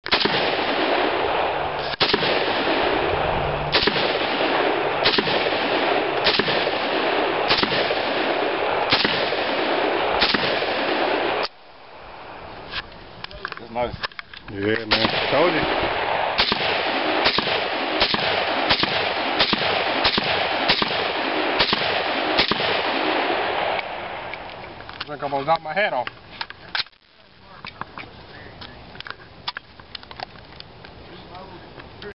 Cetme 308 vs Ak 47
Ak 47 or Cetme .308?? Which would you prefer?? Leave your comment why.

if you would like to purchase guns like these.